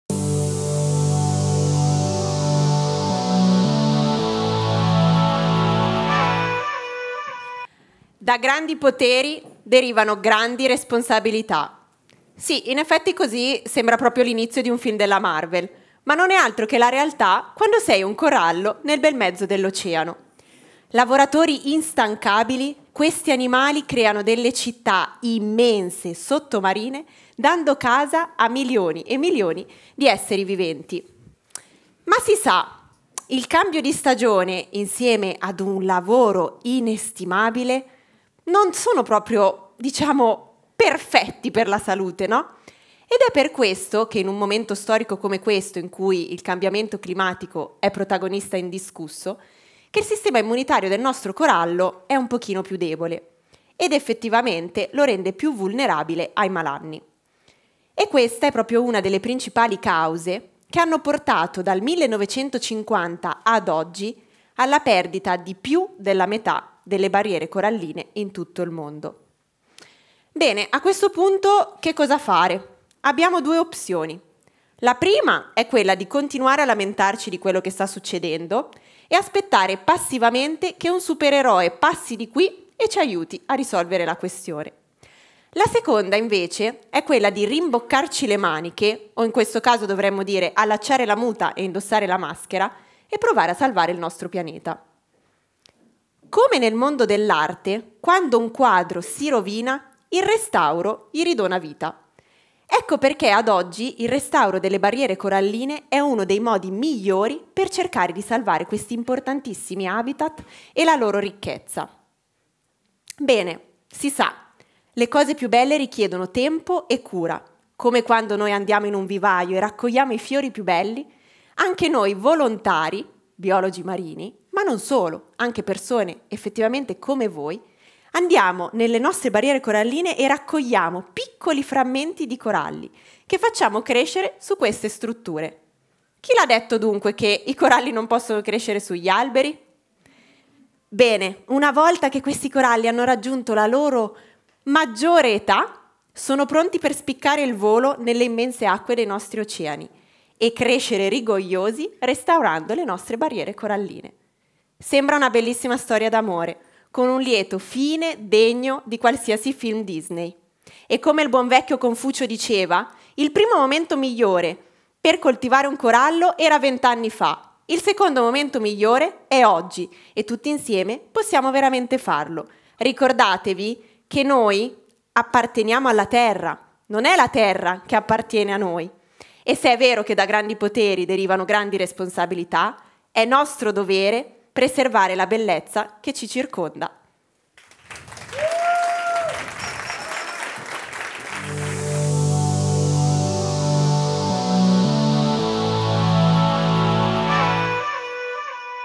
Camilla Rinaldi – Finale FameLab 2023 Genova (talk 2)
Il secondo talk di Camilla Rinaldi, finalista di FameLab Genova, è il nono della serata al Teatro La Claque del 13 aprile 2023.